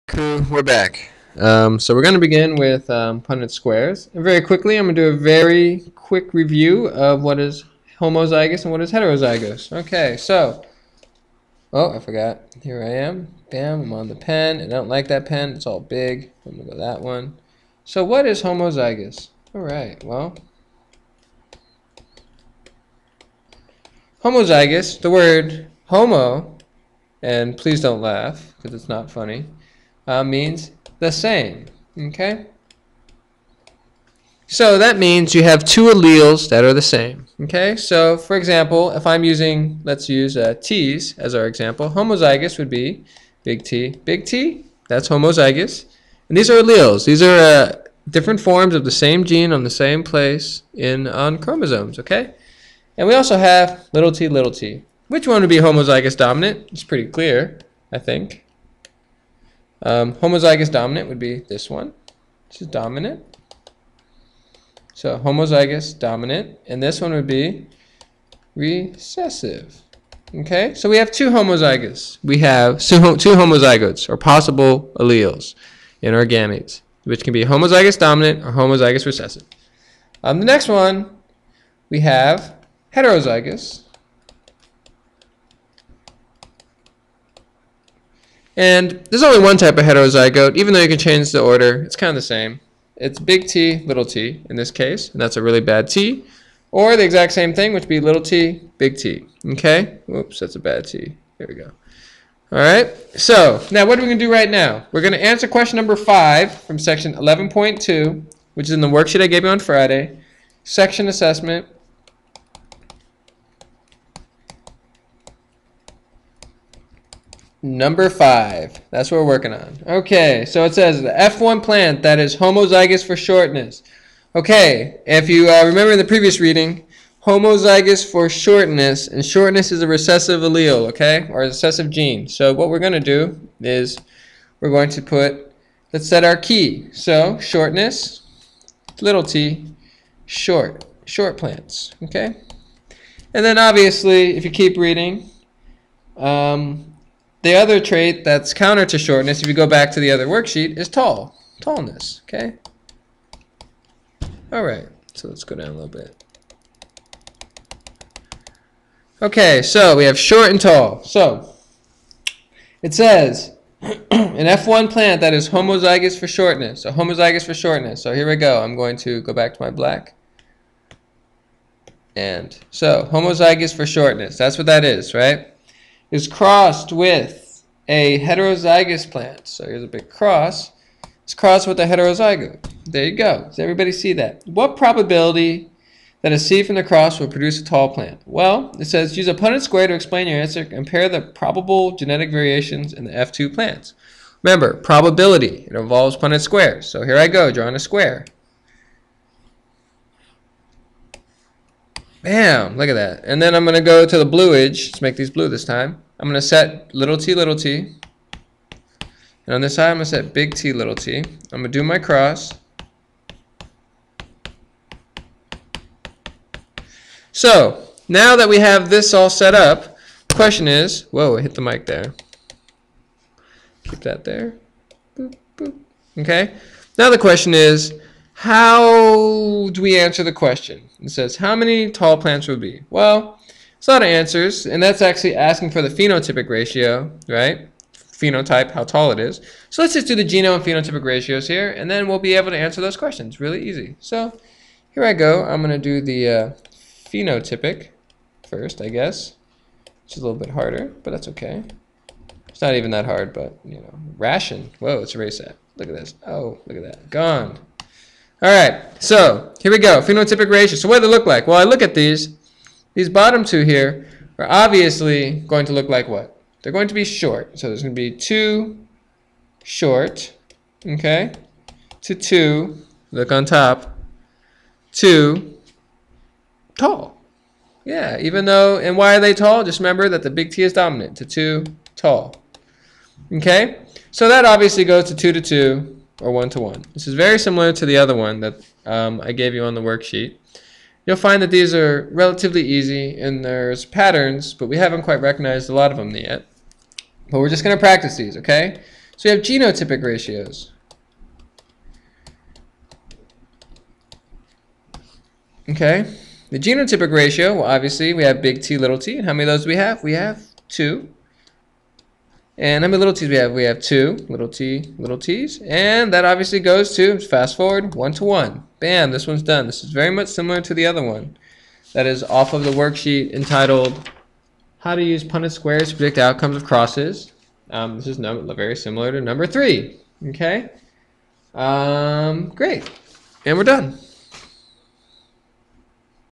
Mon 3/3: 11- 2 Section Assesment #5
This problem is very much the same as the last one.  Make sure that you can tell the  difference between homozygotes and heterozygotes.